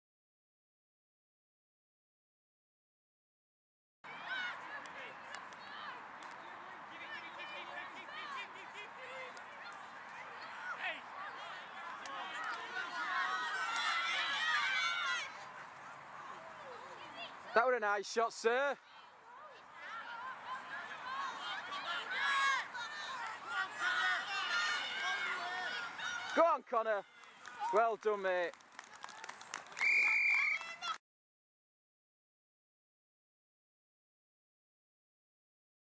Connor Wood Greatest Rugby League Try Ever!!
Connor Wood of Oulton Raiders U11's scores a magnificant try slicing through 8 defenders! Share a peice of the magic, share, favorite and rate please.